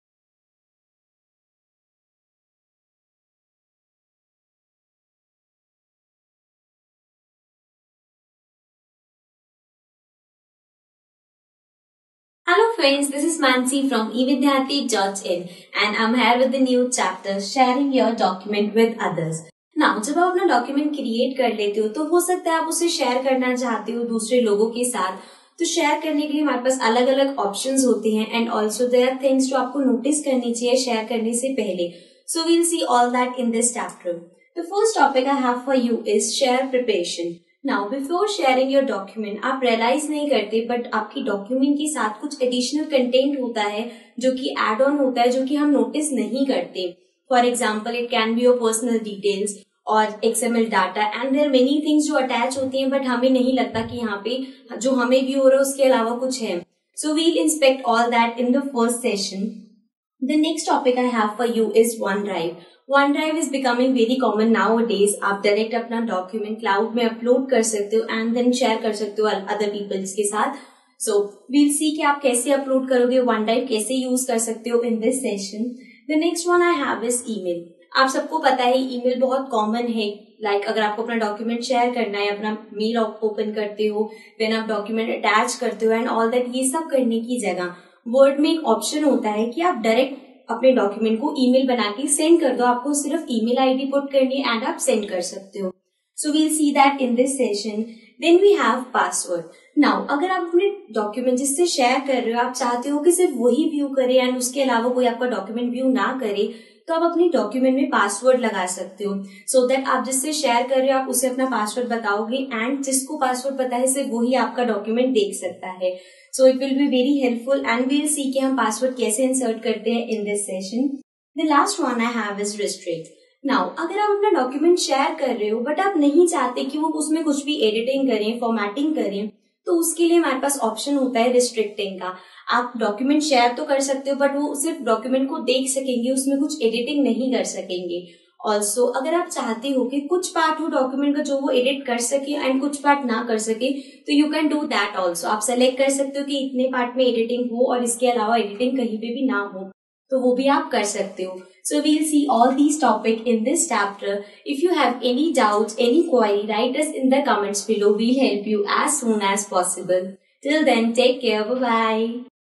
Microsoft Word 2013: Sharing Your Document - Chapter 13 Introduction
to view Microsoft Office Complete course, also watch other interesting free courses.

Explore features while Sharing Your Document in MS Word:
1.) Preparation Before Sharing
2.) One Drive
3.) Email Document
4.) Password To Your Document
5.) Restrict Your Document